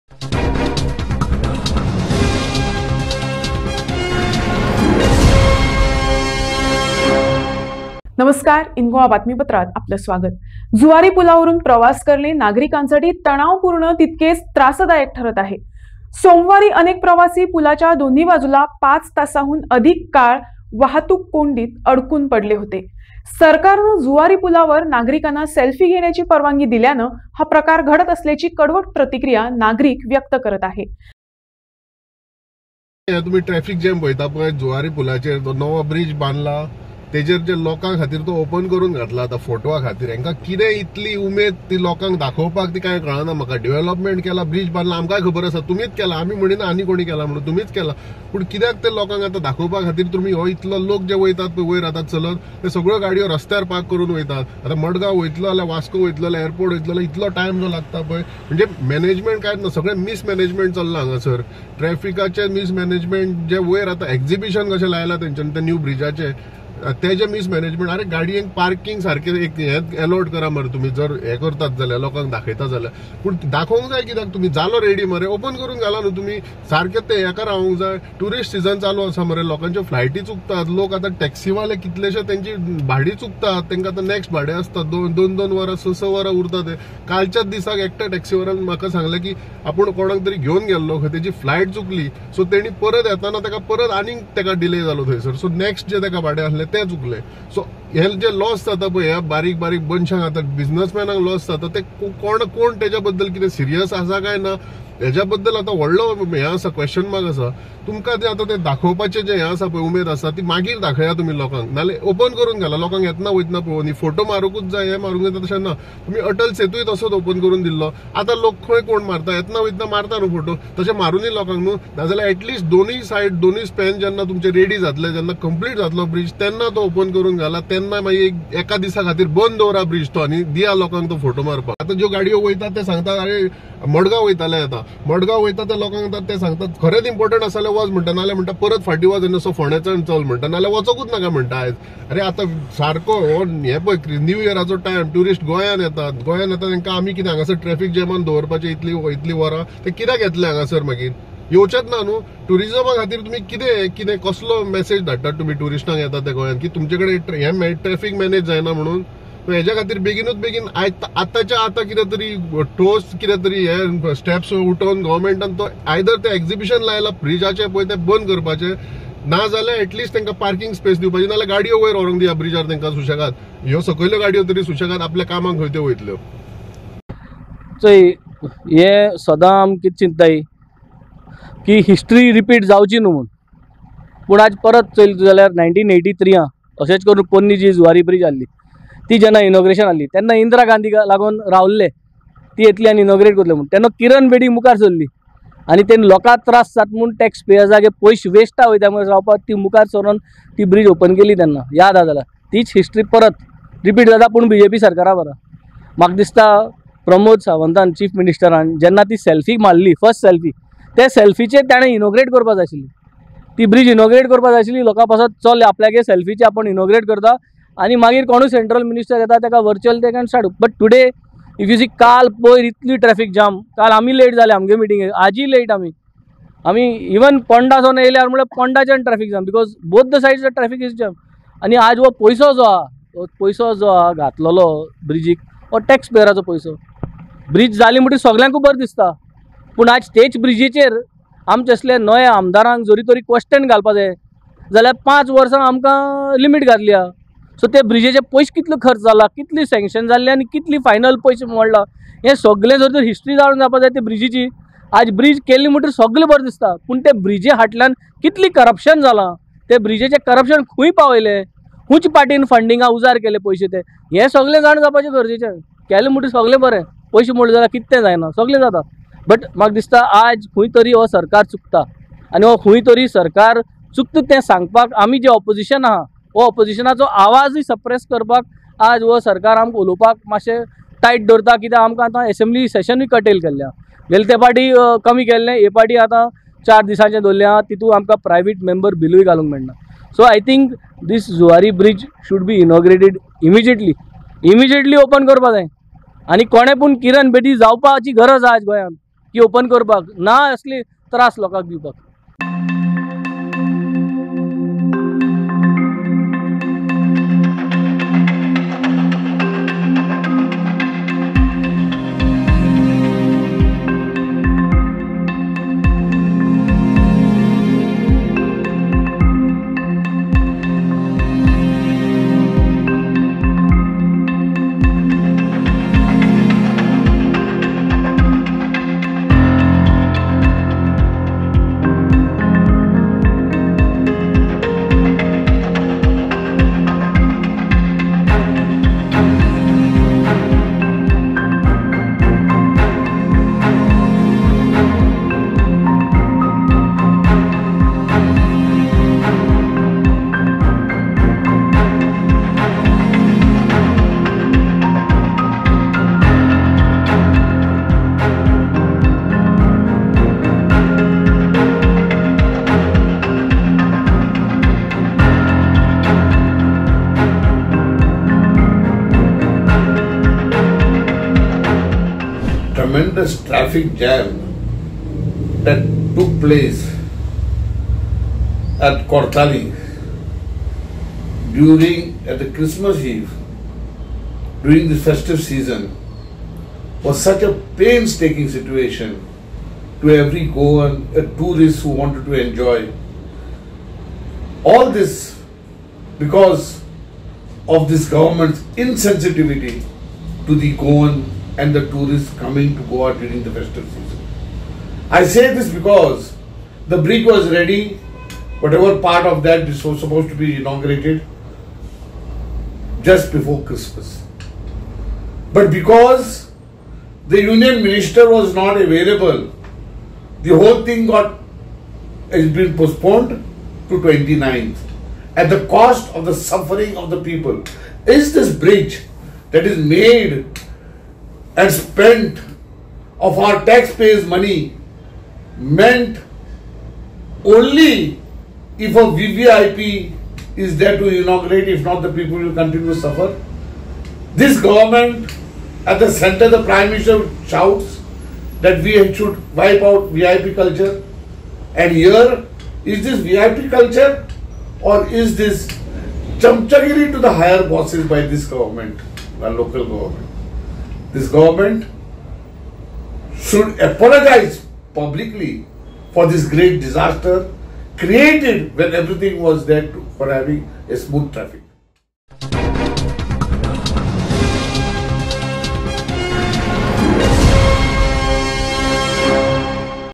Massive traffic jam on the Zuari Bridge as the stretch is become 'Selfie Zone' by the pedestrians
Traveling on the Zuari Bridge has become stressful as well as troublesome for the citizens. On Monday, many commuters were stuck in traffic jams on both sides of the bridge for more than 5 hours. Citizens are reacting bitterly that this is happening because the government has allowed citizens to take selfies on the new Zuari Bridge. Due to this, the problem of traffic congestion has become serious